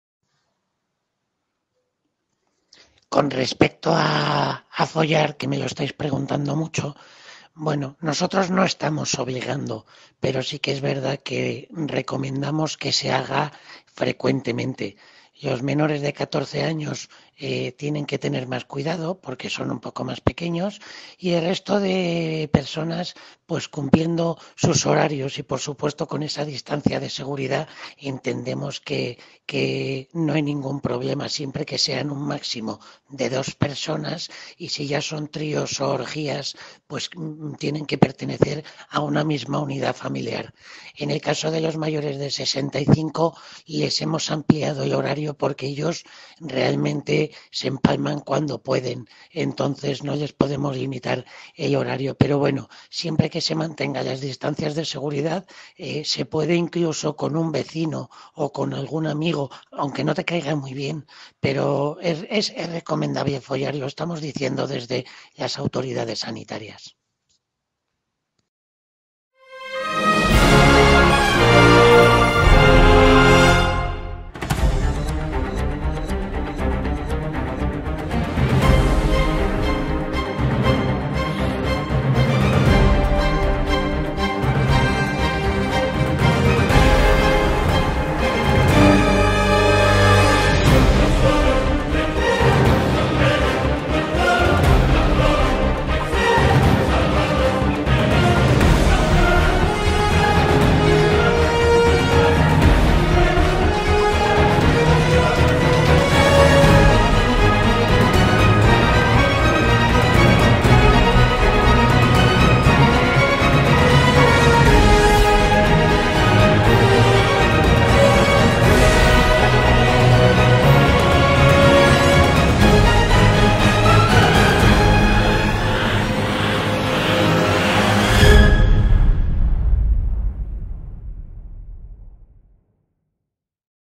"CORONAVIRUS" Fernando Simón Sanidad informa de la situación, horarios y reuniones ente personas.
El panorama se pone muy feo